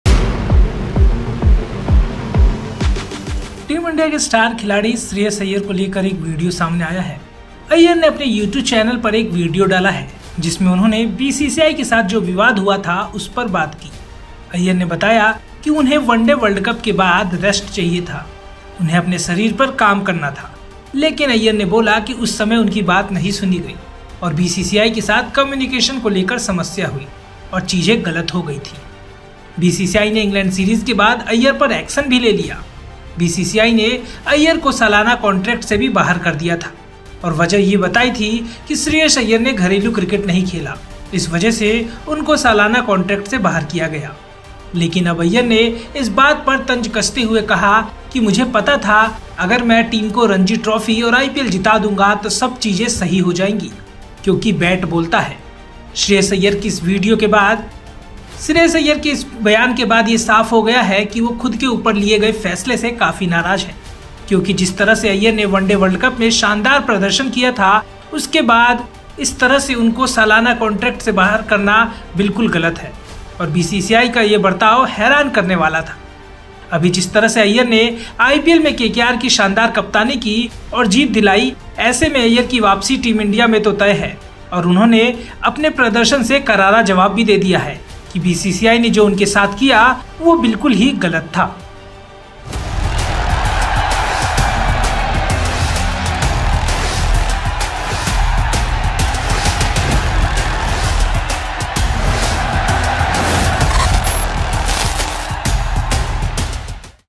Shreyas Iyer ने BCCI पर कसा तंज! टीम से निकालने पर भड़का Star Player, Contract मामले पर तोड़ी चुप्पी
Shreyas Iyer ने BCCI के साथ हुए विवाद पर अपनी चुप्पी तोड़ दी है, Iyer ने बोला WC में अच्छा प्रदर्शन करने के बाद भी Team से निकाल दिया गया